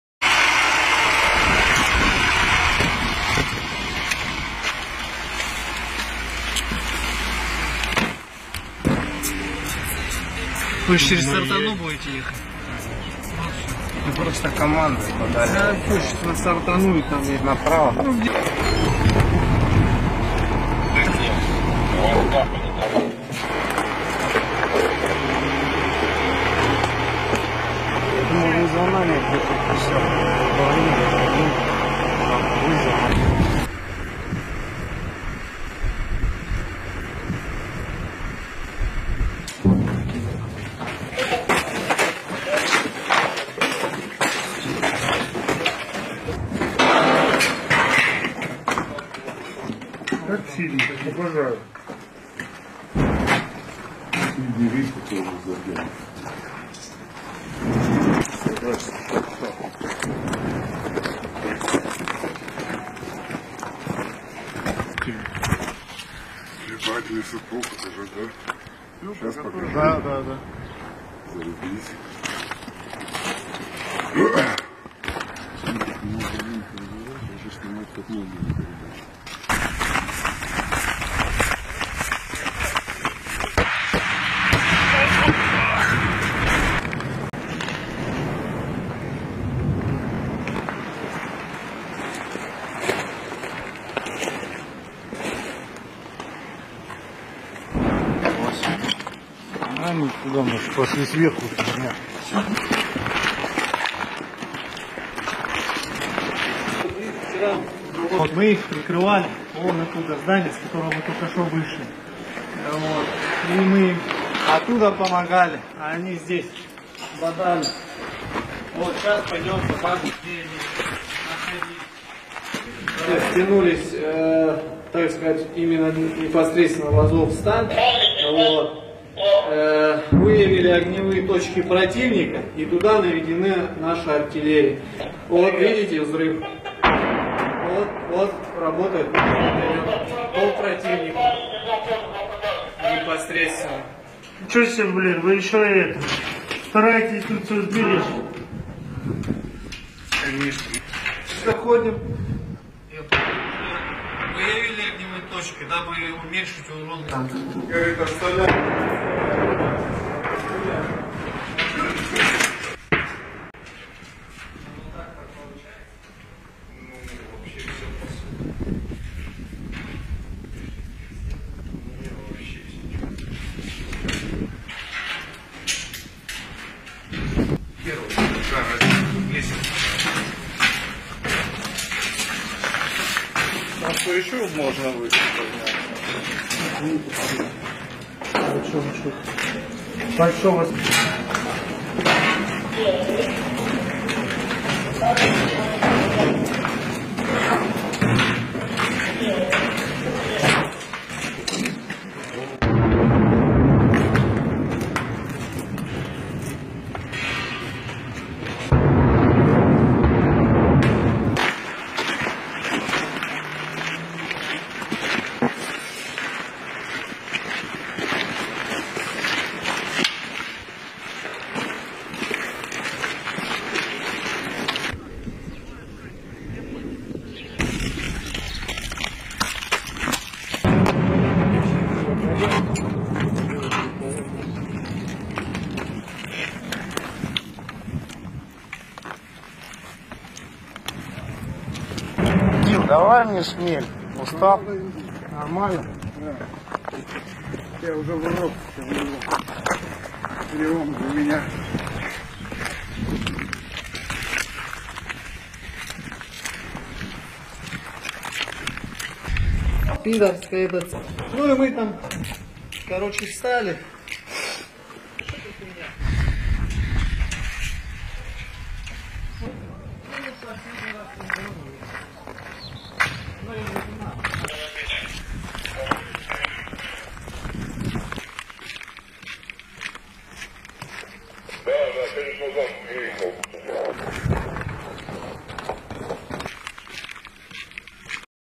Russian Special Forces clear Azov Battalion in Mariupol steel plant
President Vladimir Putin has ordered Russian special forces to block ukrainian forces in the besieged port city of Mariupol.

Putin told Russian special forces to cancel plans to storm the azovstal steel plant, where Ukrainian forces and azov battalion are still fighting back.

An azovstal iron and steel plant, a 4-square-mile (10 km) square plant, in the southeast of the city.
This place has been the last center of the Ukrainian resistance in mariupol.